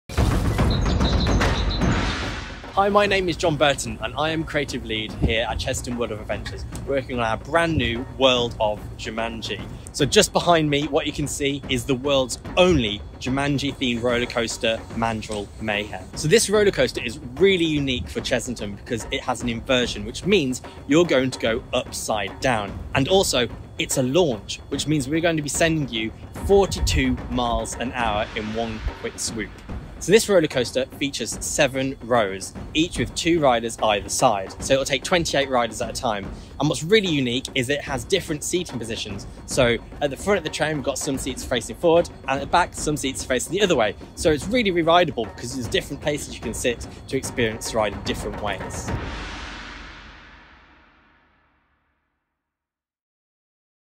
World of Jumanji at Chessington World of Adventures (New for 2023)
Chessington World of Adventures Resort has announced full details World of Jumanji - set to open in spring 2023 with the world’s only Jumanji roller coaster.